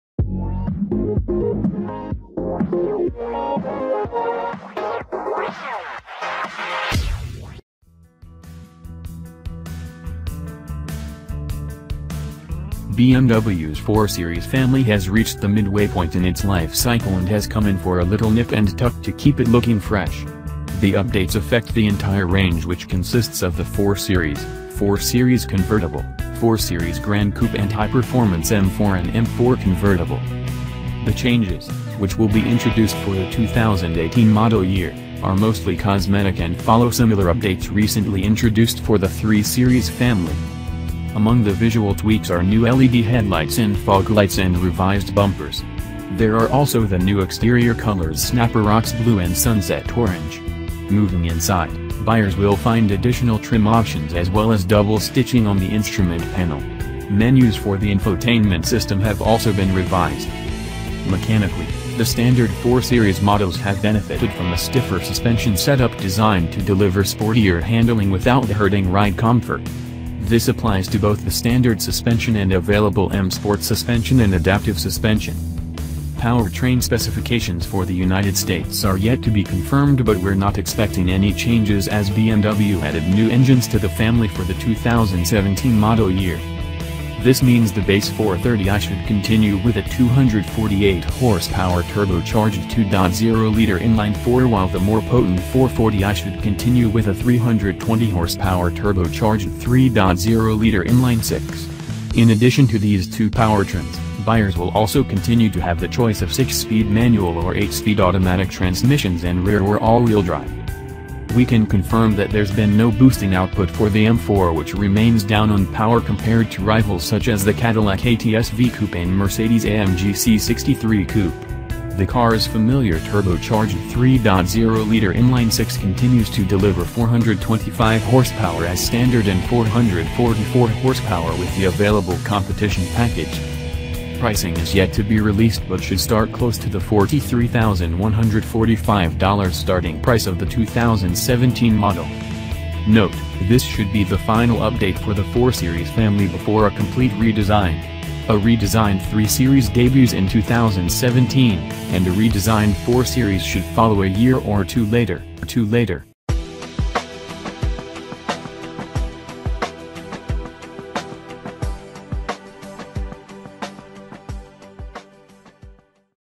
New car review | 2018 BMW 4 series preview
New car review | 2018 BMW 4 series preview.BMW’s 4-Series family has reached the midway point in its life cycle and has come in for a little nip and tuck to keep it looking fresh